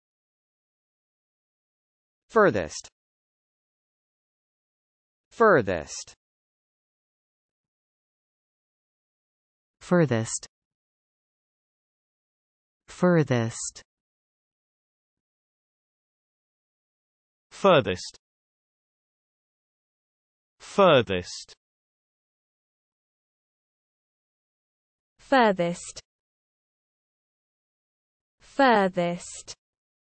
How to pronounce furthest
Learn how to say Furthest in English correctly with TextToSpeech.io free pronunciation tutorials. American & British English pronunciation of male & female voices provided in videos. Check them out!

    Furthest definitions:
    (s): (comparatives of `far'') most remote in space or time or order; "had traveled to the farthest frontier"; "don''t go beyond the farthermost (or furthermost) tree"; "explored the furthest reaches of space"; "the utmost tip of the peninsula"
    (r): to the greatest distance in space or time (`farthest'' is used more often than `furthest'' in this physical sense); "see who could jump the farthest"; "chose the farthest seat from the door"; "he swam the furthest"
    (r): to the greatest degree or extent or most advanced stage (`furthest'' is used more often than `farthest'' in this abstract sense); "went the furthest of all the children in her education"; "furthest removed from reality"; "she goes farthest in helping us"